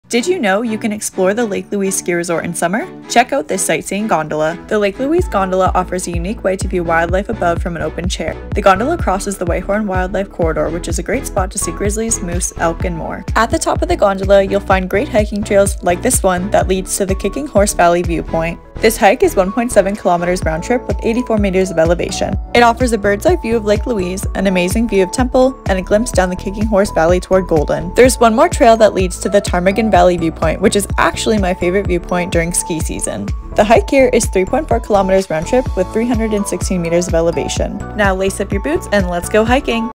Lake Louise Summer Sightseeing Lift & Gondola
Taylor, the Holisticbackpacker takes a summer journey up the Lake Louise summer sightseeing lift for a day of adventure. @theholisticbackpacker6114